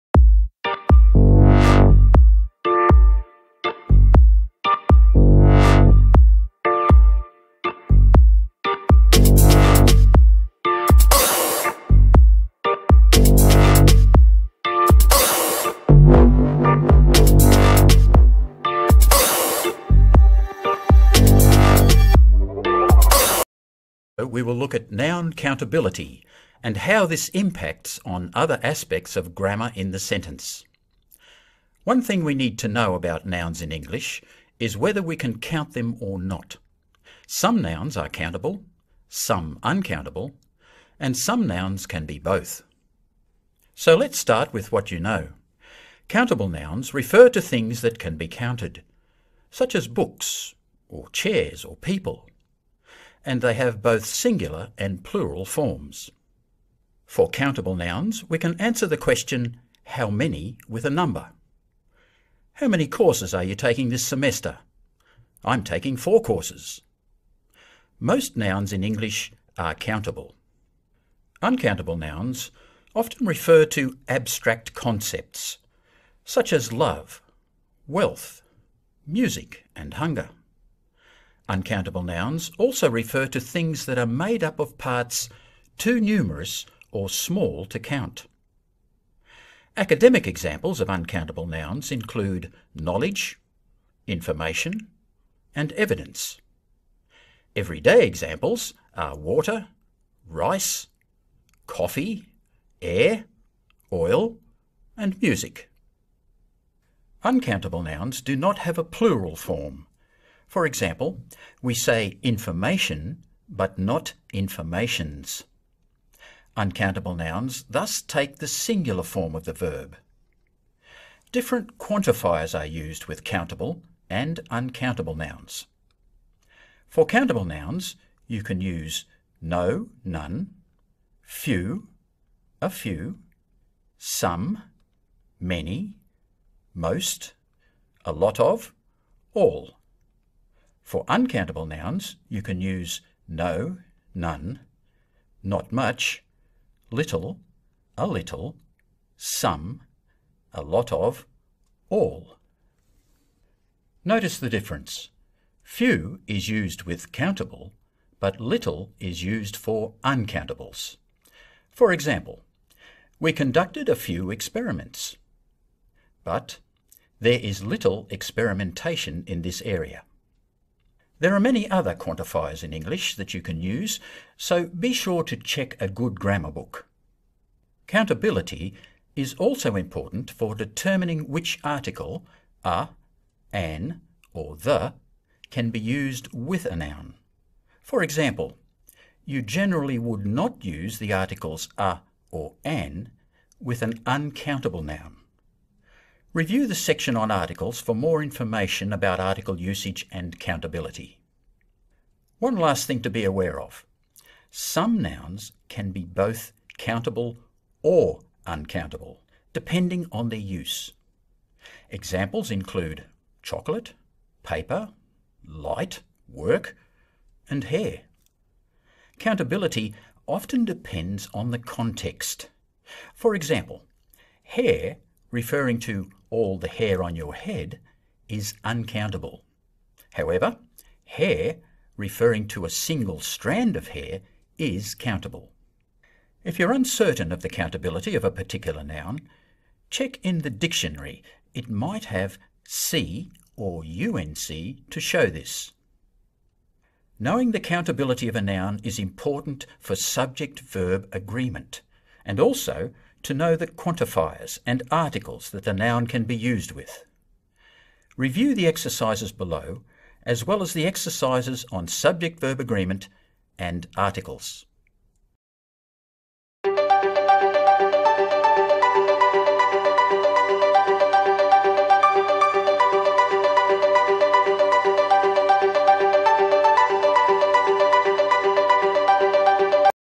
Learn English Grammar Countable and Uncountable Nouns
In this video, we will look at noun countability and how this impacts other aspects of grammar in the sentence. One thing we need to know about nouns in English is whether we can count them or not: some nouns are countable, some uncountable and some nouns can be both.